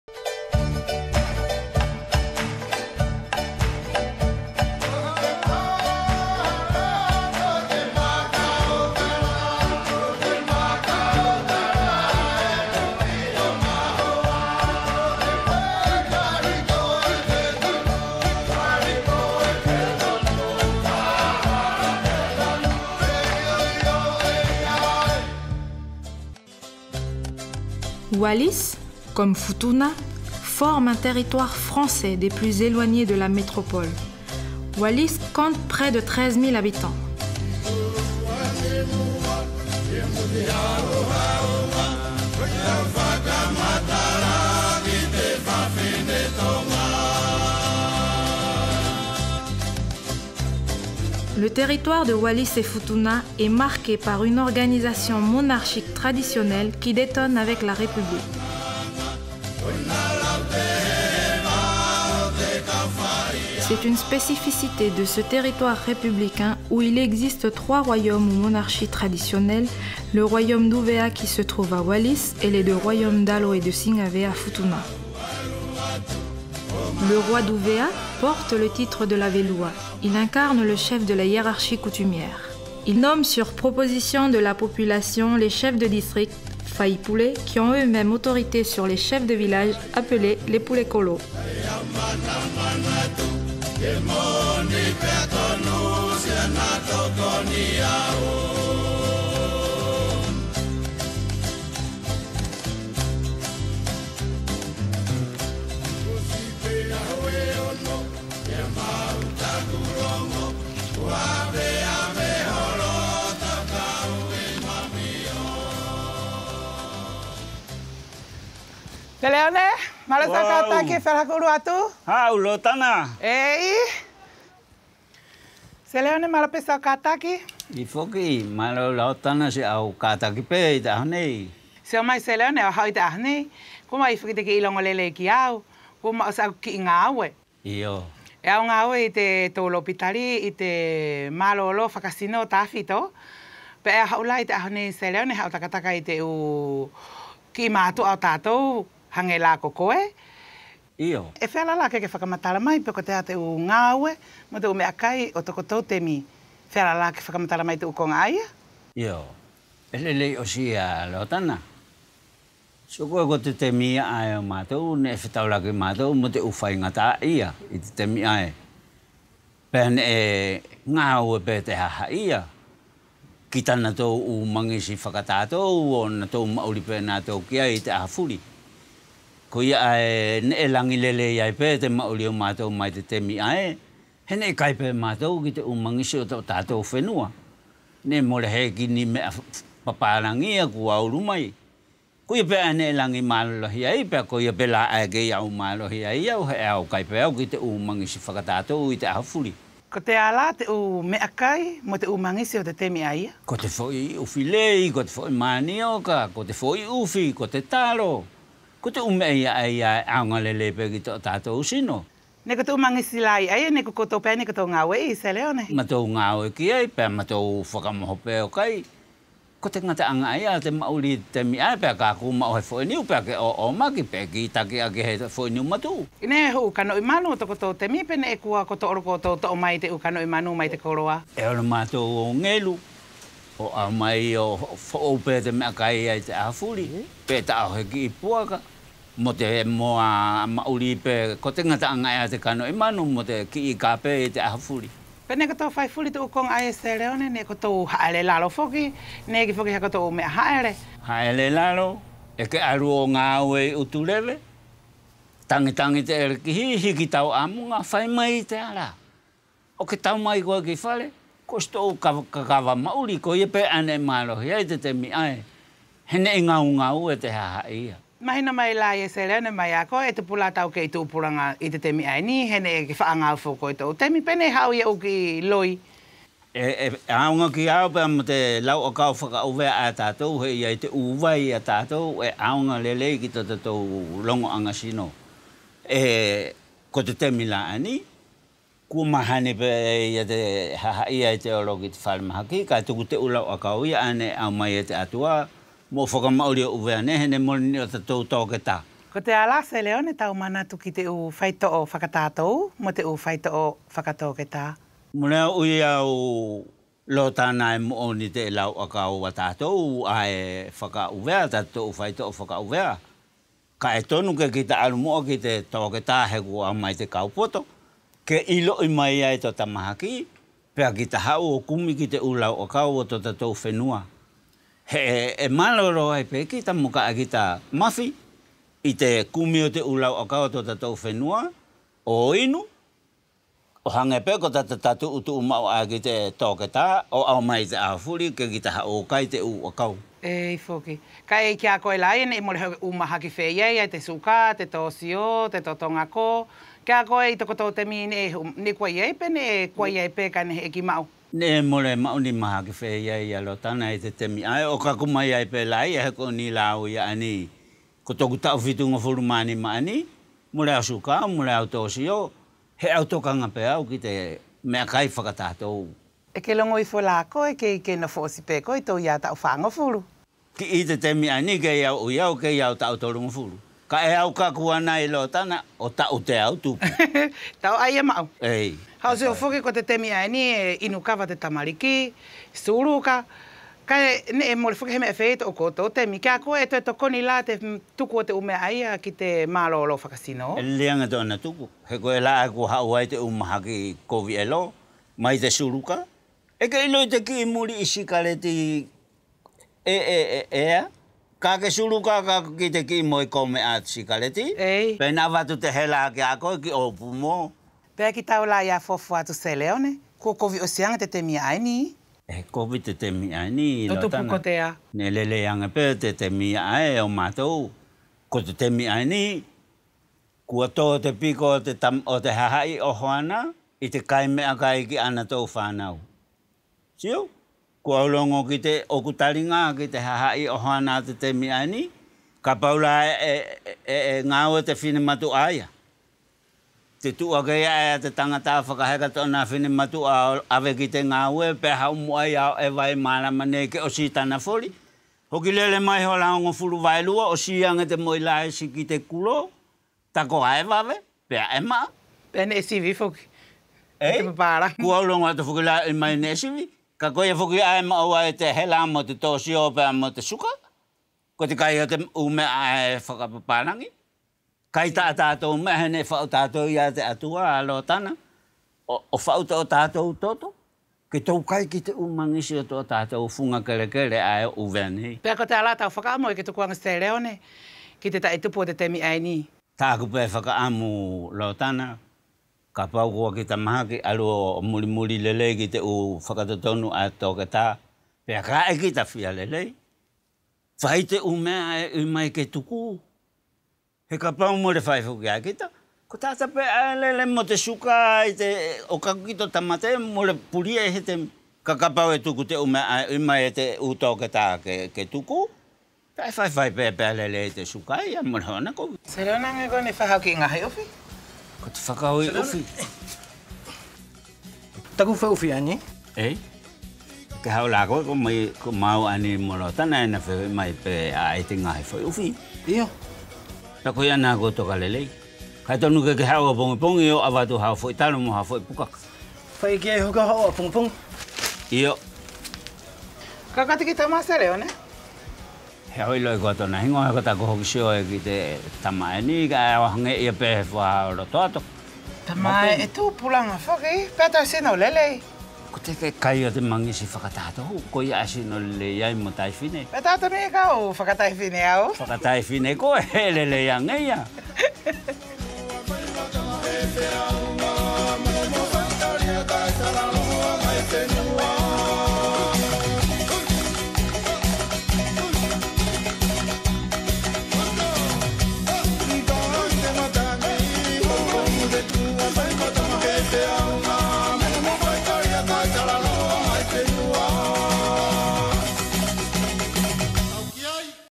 Wake up! Team Wallis & Futuna - "Seleone"
This video is part of the Wake up! Project, designed to mobilise the region’s young people (18-25 years old) on the non-communicable diseases issue and use more dynamic communication techniques to deal with this health topic.
Video description: Meet Seleone, an elder from Hihifo district on Wallis Island. He is interviewed by Lotana, a nurse, who asks him about traditional ways of life, and how to integrate this knowledge into methods of teaching young people about non-communicable diseases. Through interviews with Seleone, the film shows how modernisation has profoundly changed the Wallisian way of life, with a focus on the abandonment of subsistence farming, hunting and fishing, and the introduction of imported processed foods.